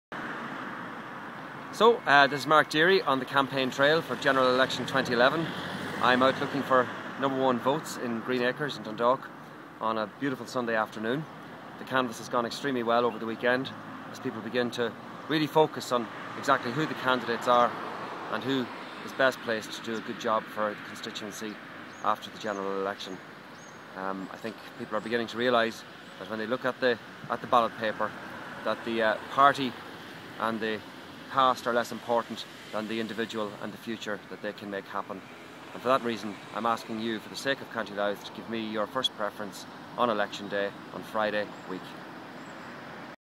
Greenacres on a sunny Sunday
Green party Candidate Senator Mark Dearey canvassing in Green Acres Dundalk Co Louth for General Election 2011